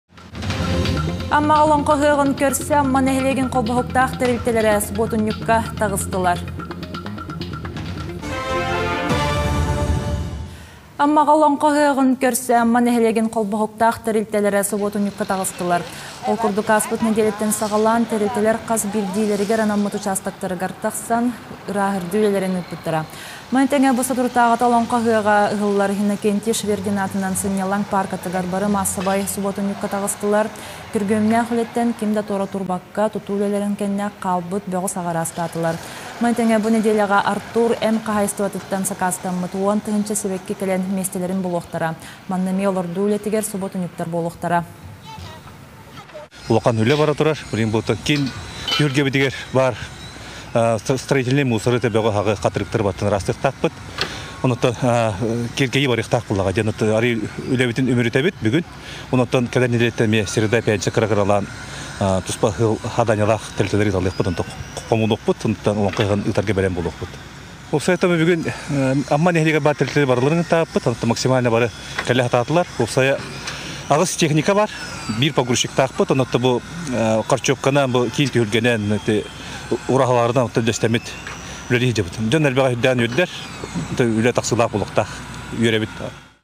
АММАҔА ОЛОҤХО ЫҺЫАҔЫН КӨРСӨ СУБУОТУННЬУККА ТАҔЫСТЫЛАР (САХА СИРЭ 03.06.2024 20:30ч)
Аммаҕа Олоҥхо ыһыаҕын көрсө Амма нэһилиэгин холбоһуктаах тэрилтэлэрэ субуотунньукка таҕыстылар